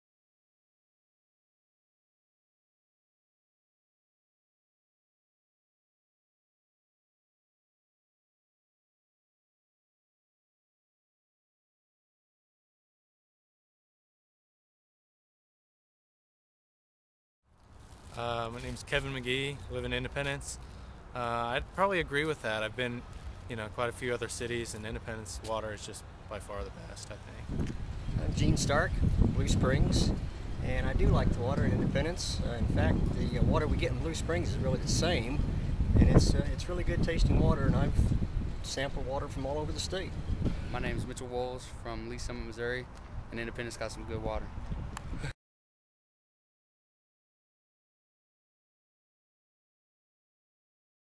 Your Take: Independence Water
The Examiner asks people on the street what their take is on Independence having the 4th best tasting tap water in the world?